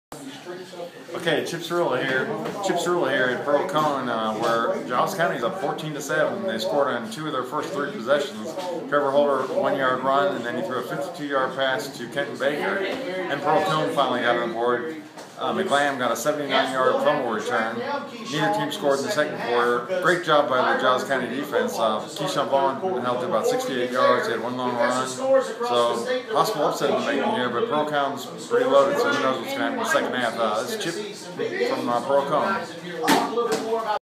Halftime Report: Giles Co. 14, Pearl-Cohn 7
Tennessean Preps Insider Chip Cirillo provides a halftime report from Giles Co. vs. Pearl-Cohn.